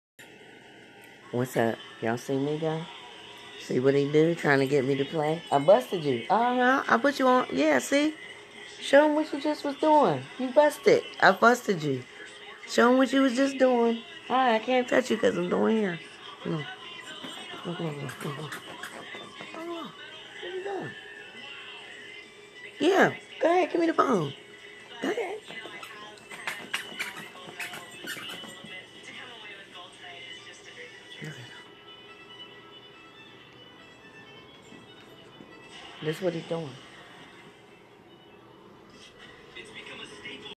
Migo spoiled rotten yorkie poo trying sooo harrrd to distract 😅
Doin hair painfree reppin